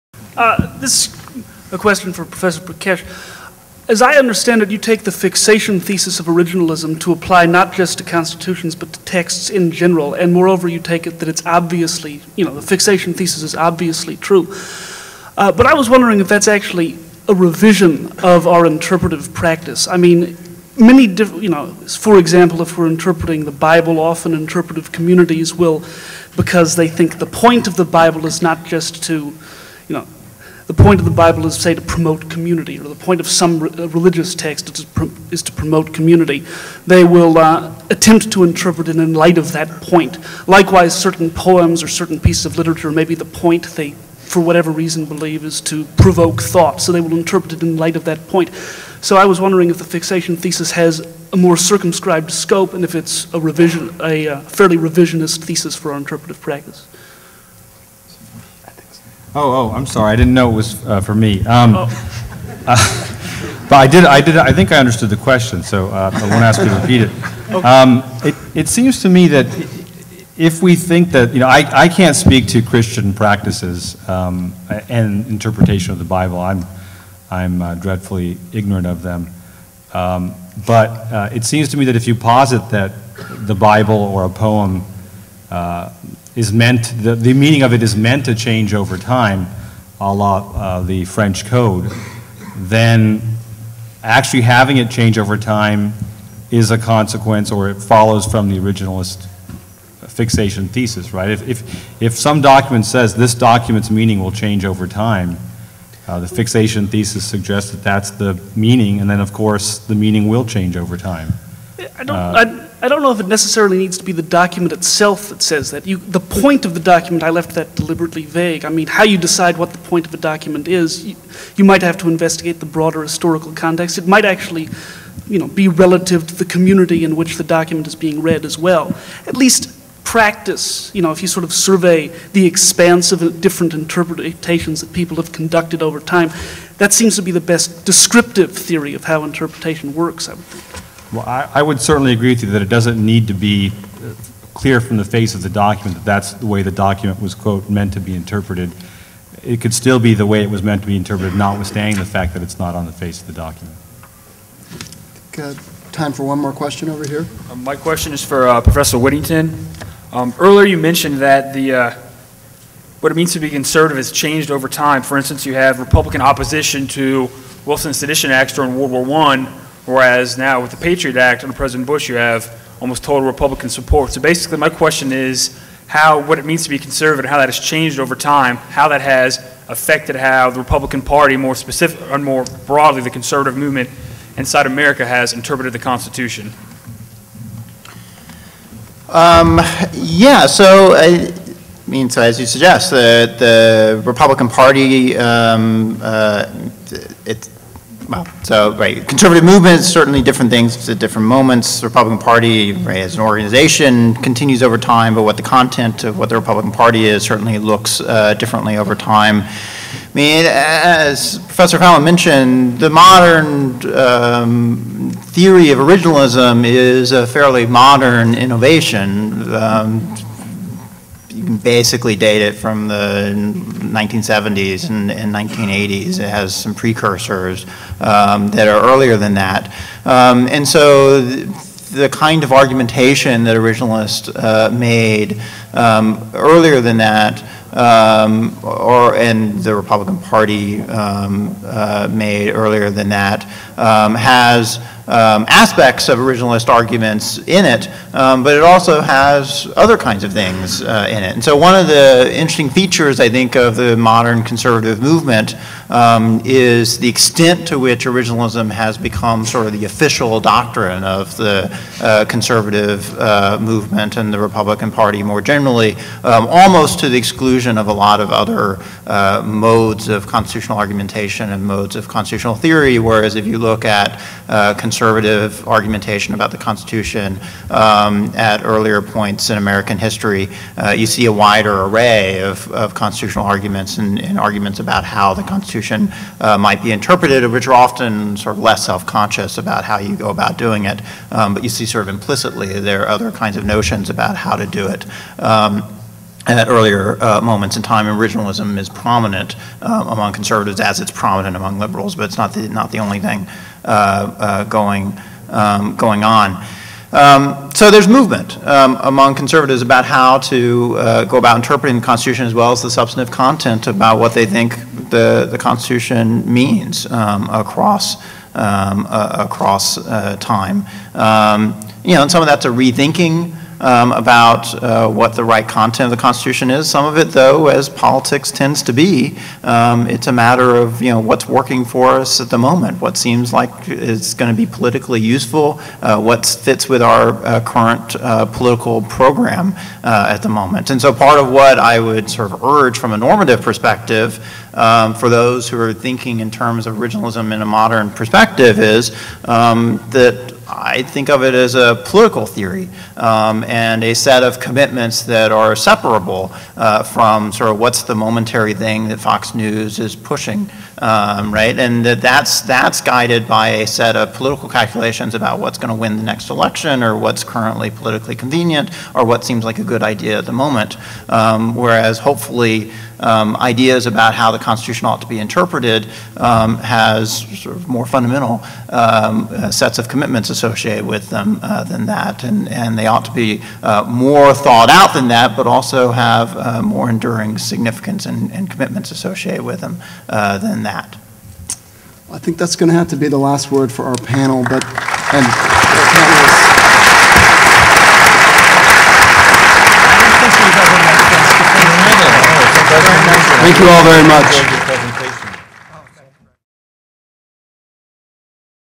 Originalism: A Rationalization or Principled Theory? Pt. 13
One of the criticisms of originalism is that it simply is code for conservative ideology.  Pointing to figures such as Justice Scalia, some characterize originalism merely as a tool of the Republican Party, suggesting that all originalist jurisprudence is also politically conservative.  The first question for originalism is what justifies originalism as a theory of constitutional interpretation as opposed to a political program. Originalists have advanced a variety of justifications for originalism. Some justify it on the basis that originalism is implicit in the use of language. Others suggest that originalism is to be preferred, because it is the theory that delivers the clearest rules. This panel will explore such justifications as well as critiques of originalism. The Federalist Society's Student Division presented this panel at the 2010 Annual Student Symposium on February 26, 2010.  Opening remarks were delivered by University of Pennsylvania Law School Symposium Director Ryan Ulloa and Dean Michael A. Fitts of the University of Pennsylvania Law School.  Panelists included Prof. Mary Anne Case of the University of Chicago Law School; Prof. Richard Fallon of Harvard Law School; Prof. Saikrishna Prakash of the University of Virginia School of Law; Prof. Keith Whittington of Princeton University; and Hon. Greg Garre, 44th Solicitor General of the United States, as the moderator. Part 13 of 13